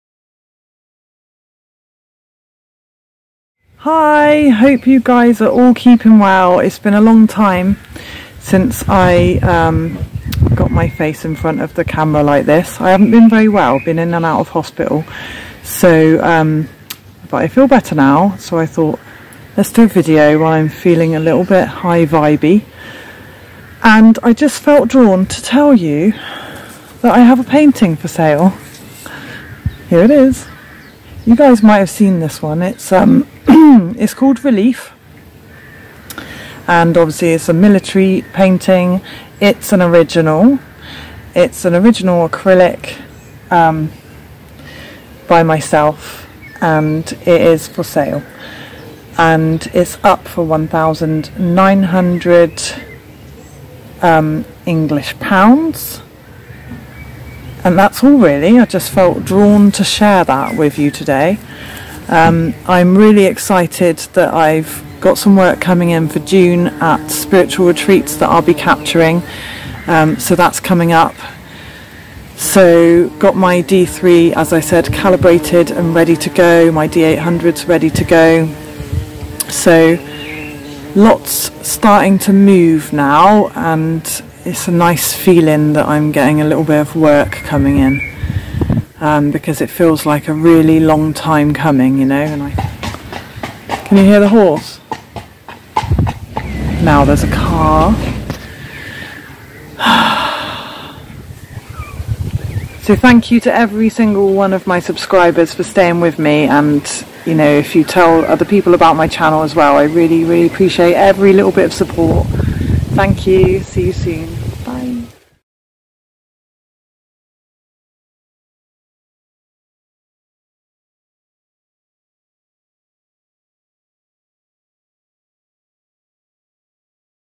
ORIGINAL ART INVESTMENT
What happened waaaas. In 2013, a van hit my beautiful Mazda 6 from behind and wrote it off. Thank goodness it was just me in the car.

My neck injury meant I was unable to lift my camera for 9 months.

All my photography and lecturing work stopped in an instant. And everything changed...

Because I couldn't lift my camera, I picked up a paintbrush. I painted Military Scenes in memory of my Grandad who lost his leg during WW2. I painted my way through my pain and frustration, I grieved and I awakened.

I trust that my Original Military Art will find where in the world it's meant to be and bring the same healing and awakening energy to whoever it belongs to.